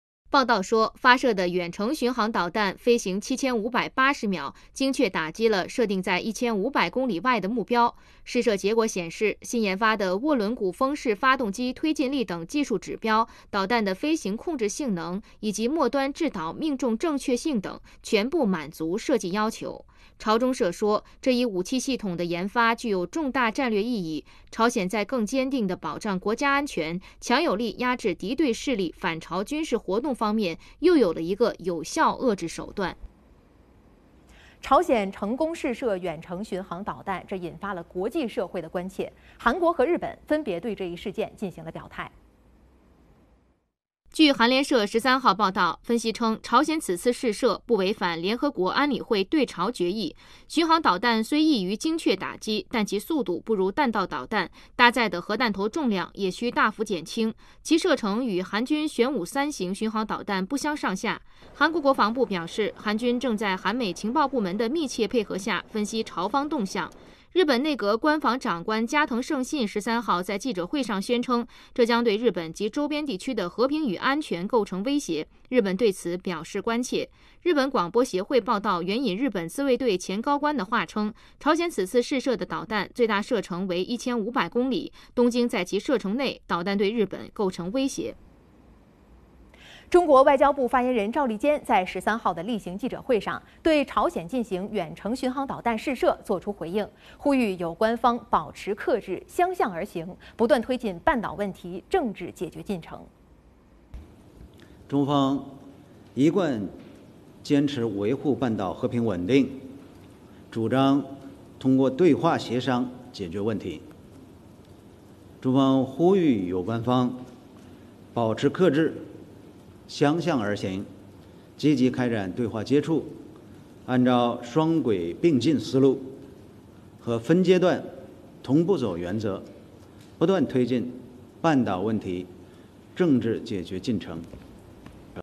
朝鲜试射远程巡航导弹引日韩关切 中方作出呼吁
据朝中社9月13日报道，朝鲜国防科学院11号和12号分别成功试射了新研发的远程巡航导弹。发射的远程巡航导弹飞行7580秒，精确打击了设定在1500公里外的目标。